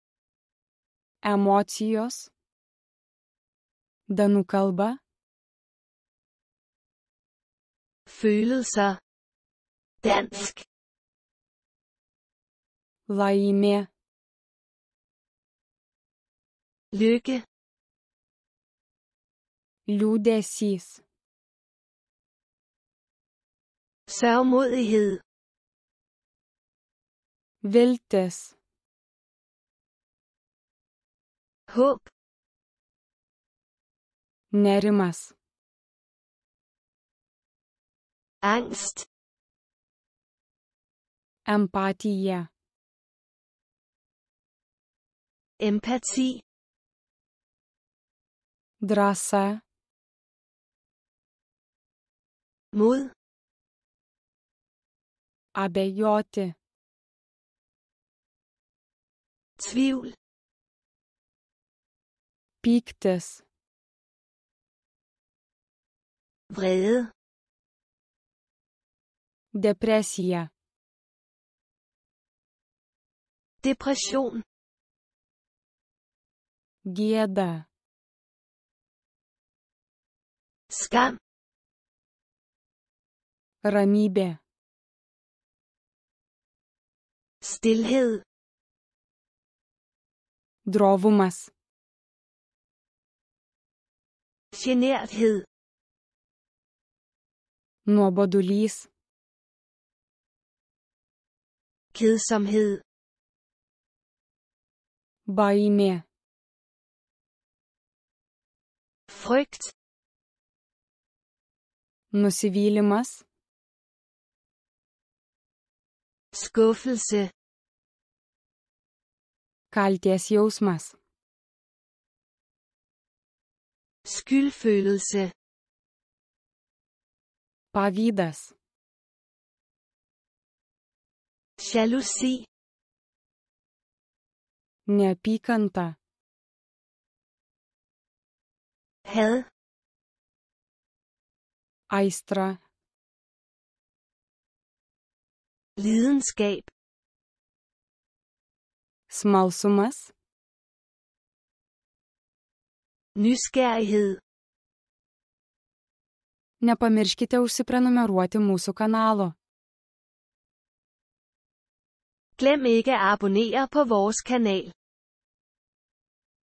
Emocijos (Danų kalba) (lt-da)
Fone TV
lt-da-11
Følelser (Dansk)
Laimė - Lykke
Liūdesys - Sørgmodighed
Viltis - Håb
Nerimas - Angst
Empatija - Empati
Drąsa - Mod
Abejoti - Tvivl
Pyktis - Vrede
Depresija - Depression
Gėda - Skam
Ramybė - Stilhed
Drovumas - Generthed
Nuobodulys - Kedsomhed
Baimė - Frygt
Nusivylimas - Skuffelse
Kaltės jausmas - Skyldfølelse
Pavydas - Jalousi
Neapykanta - Had
Aistra - Lidenskab
Smalsumas - Nysgerrighed